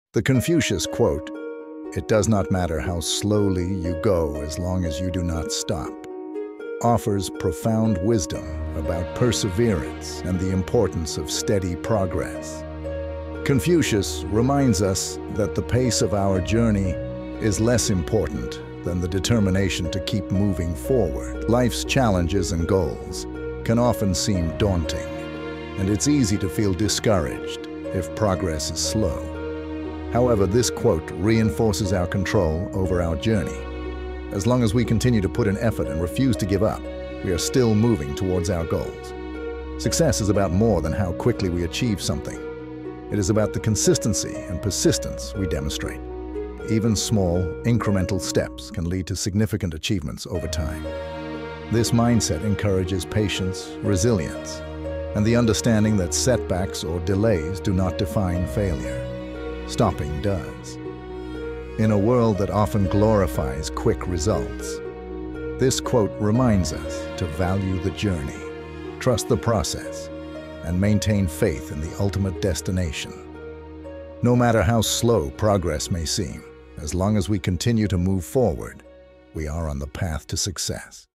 Confucius on Perseverance Slow Progress, Steady Growth, and Success Inspirational Quotes
Explore Confucius's wisdom and the importance of perseverance. Learn why steady progress, even if slow, leads to success as long as you never give up. This video offers inspiration and motivation for those facing challenges on their journey.